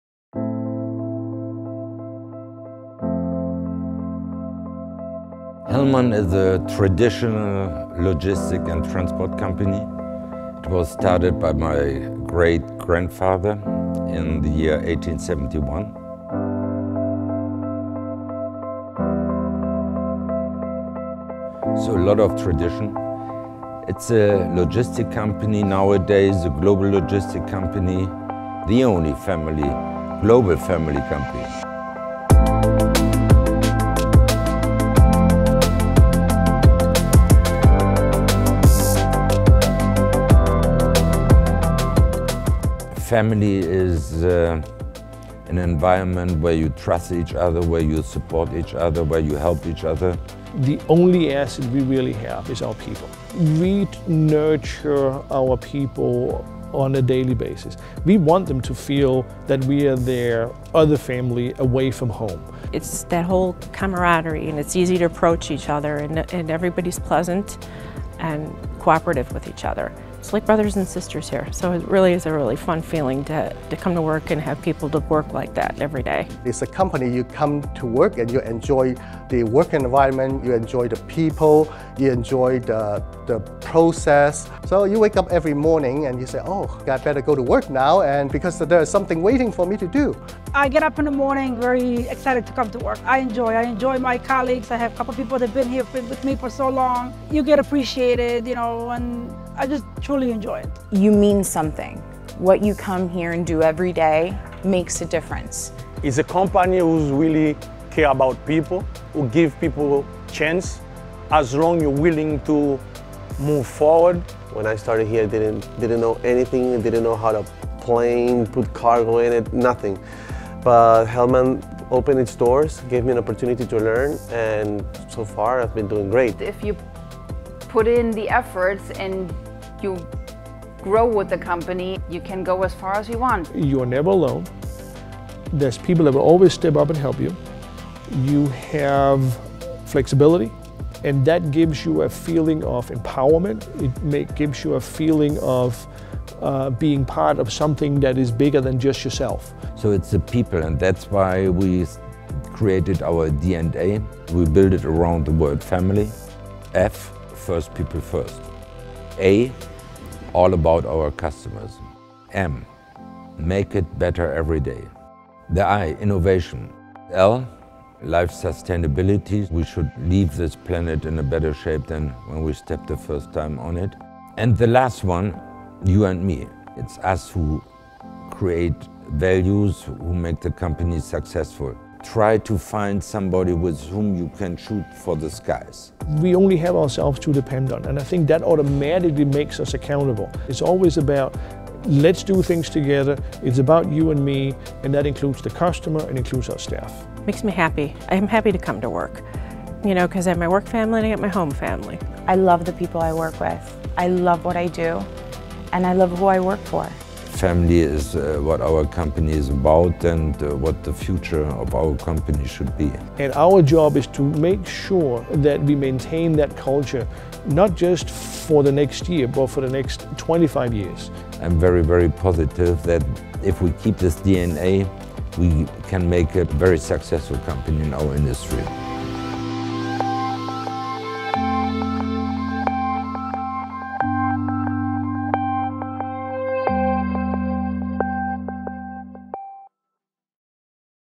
FAMILY at Hellmann Worldwide Logistics
Founded in 1871, our company started with one man, Carl Heinrich Hellmann, using a horse-drawn cart to deliver parcels in and around the town of Osnabrueck, northern Germany. Four generations later, Hellmann is a company with a worldwide network of 20,500 people in 489 branches in 173 countries.